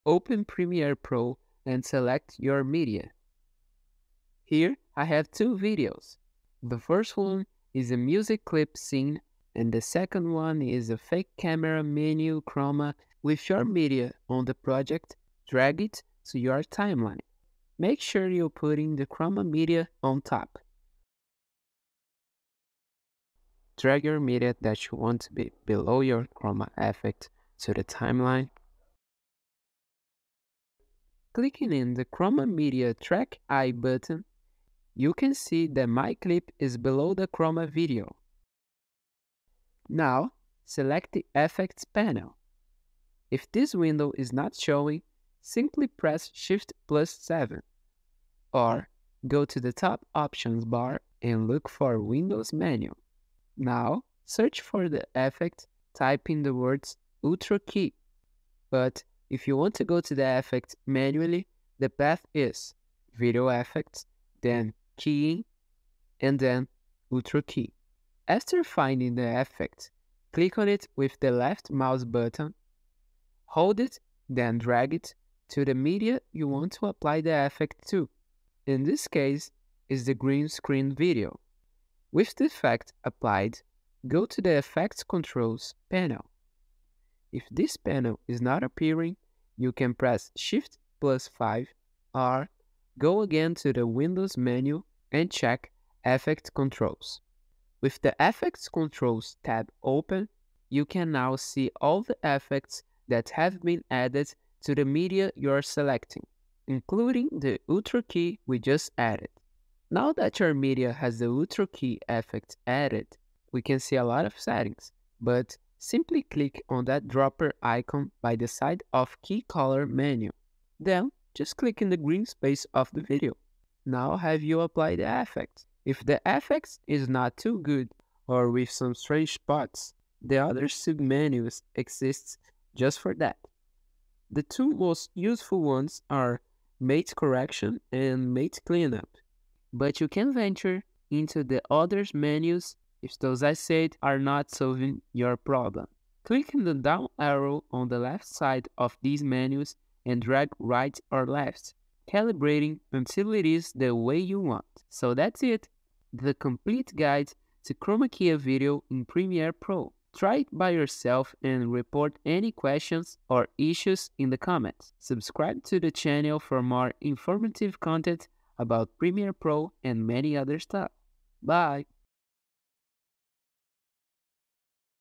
How to Remove Green Screen in Premiere Pro for Beginner
In this video, we're going to show you how to remove green screen in Premiere Pro for beginners.

If you're looking to add some custom video effects to your videos, then you'll want to watch this video! We'll show you how to remove green screen from your footage in Premiere Pro, so that you can create unique and exclusive videos that will impress your viewers!